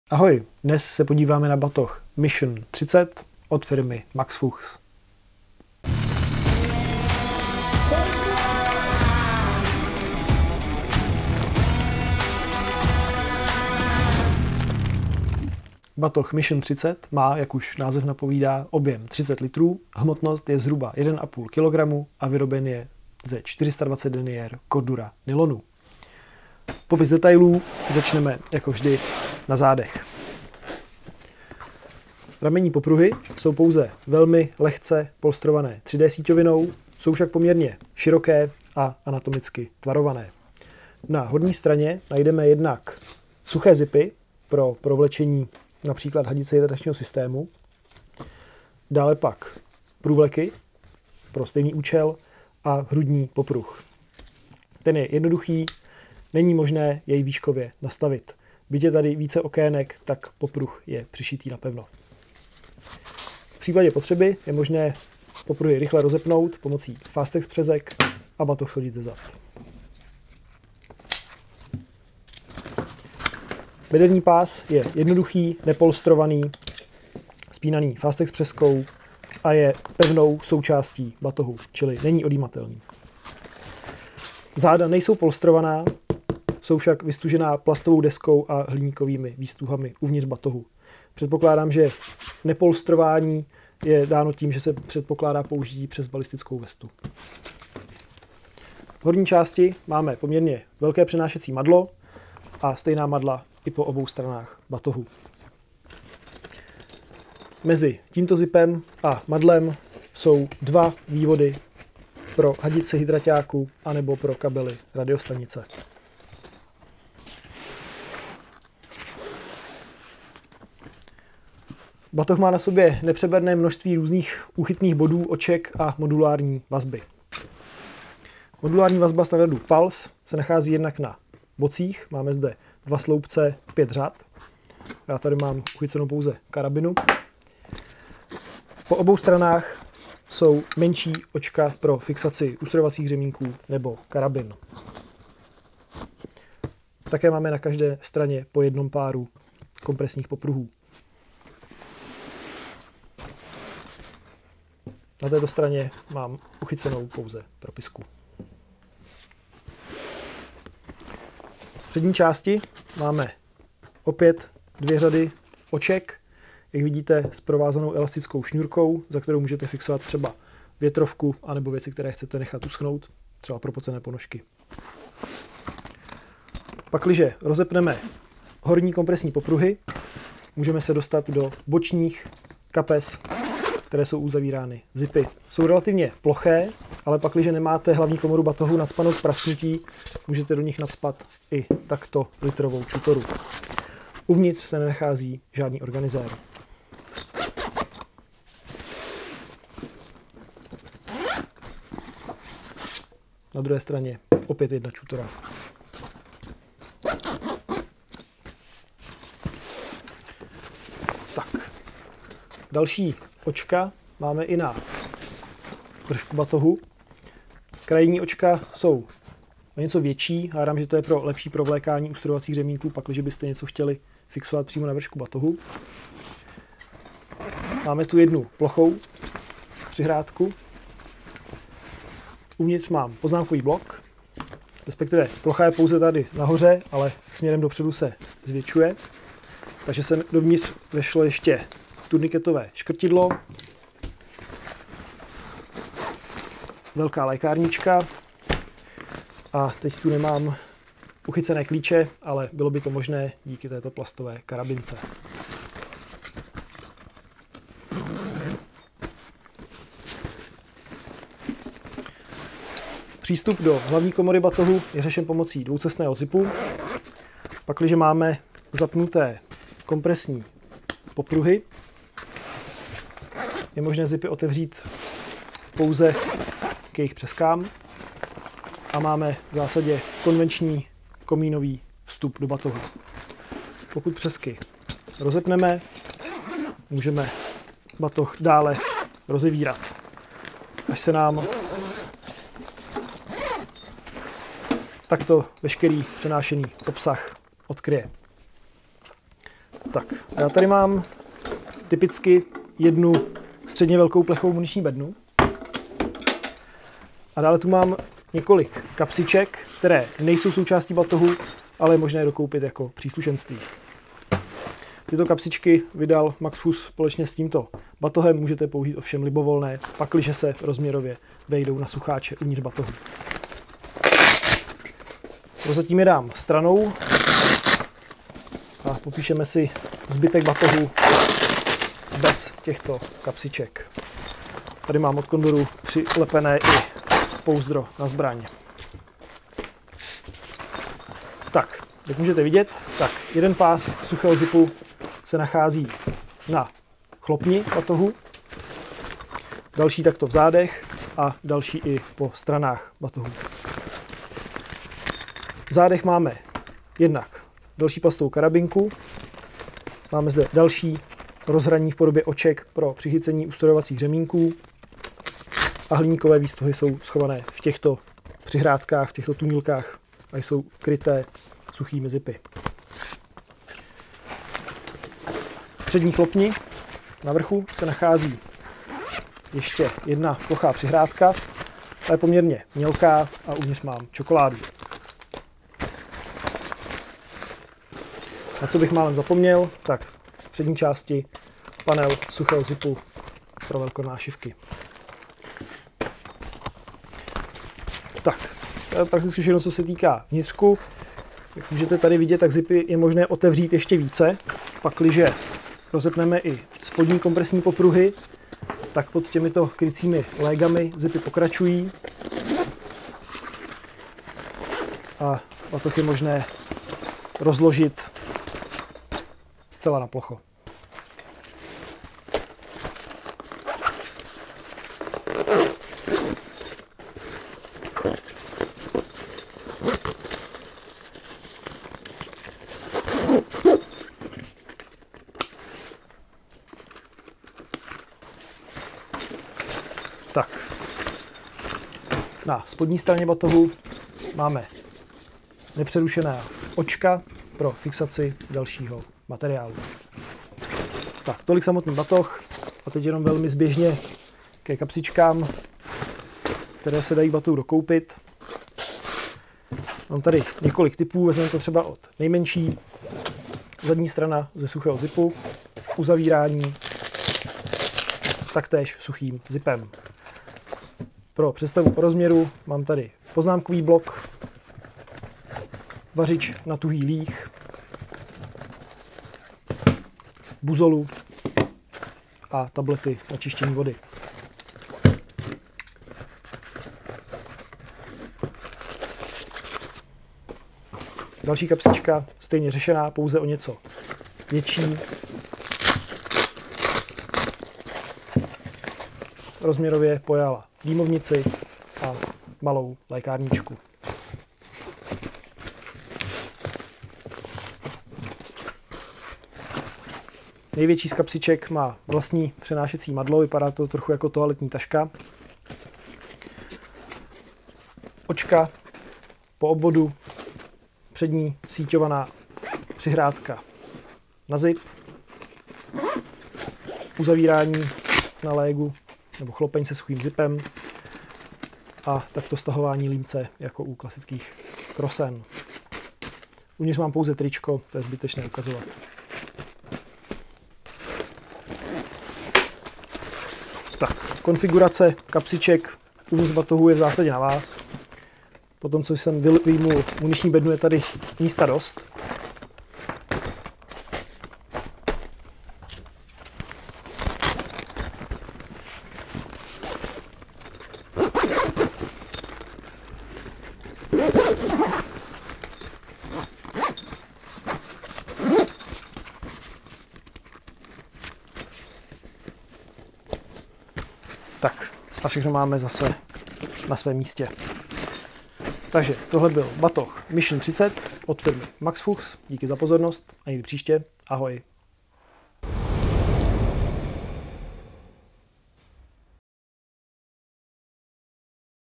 Batoh MFH MISSION 30 - Military Range CZ/SK
Batoh nabízí:

- nastavitelné, polstrované popruhy se smyčkami
- nastavitelné hrudní a pánevní popruhy
- rukojeťi na snadné přenášení /mezi zádovými popruhy a na bocích/
- kapacita: cca 30 l
- rozměry: cca 30 x 55 x 25 cm (W x H x D)
- hmotnost: cca 1,5 kg
- materiál : 100% nylon 420D (Cordura)